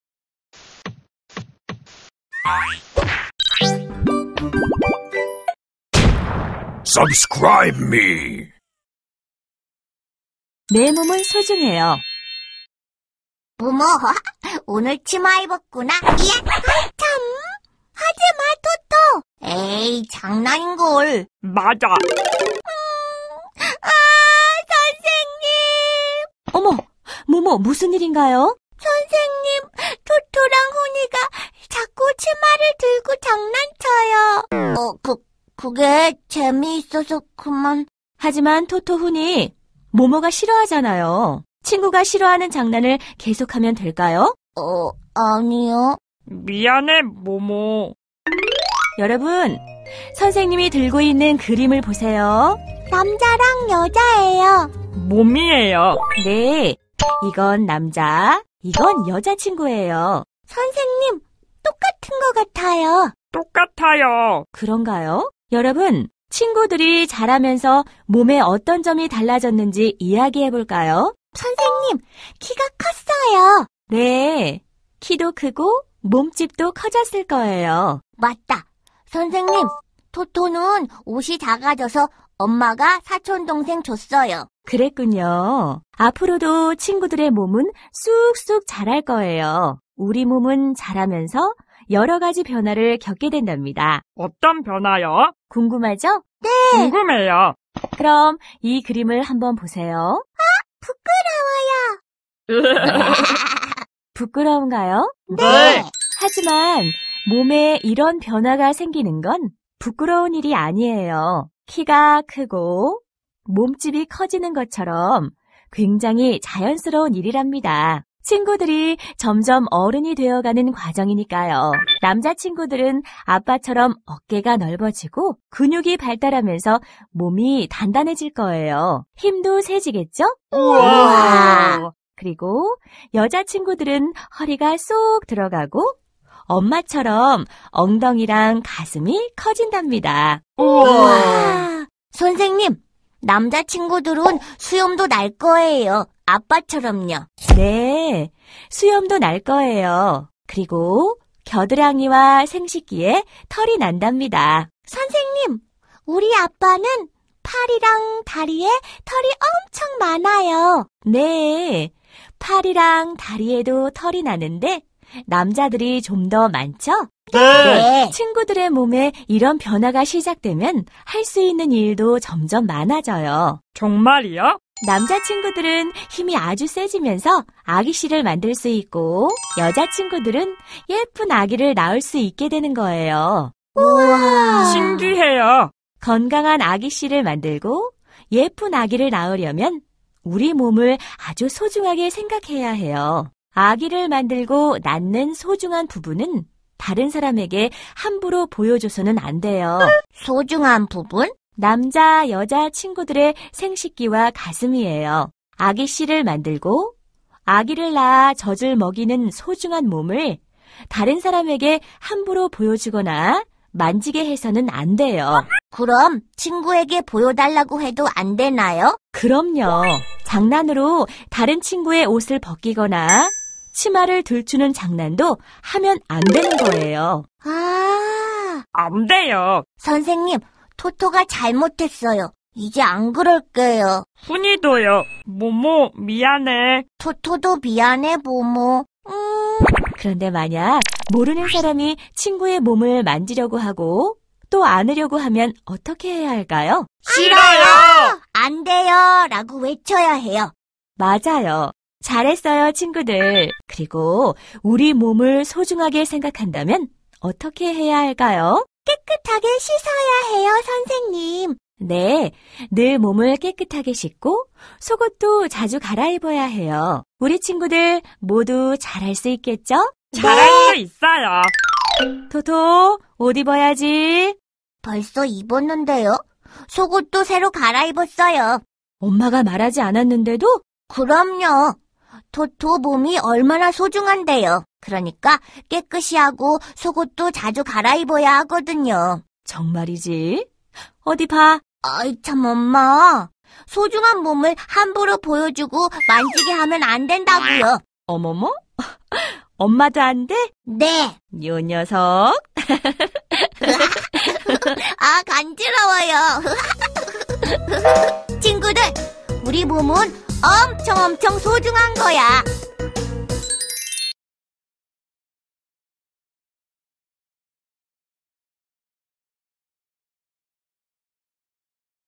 내 몸은 소중해요 | 토토의 생활안전 | 깨비키즈 KEBIKIDS
우리 몸의 성장 과정에 대해 알고, 몸의 소중한 기관에 대해 공부합니다.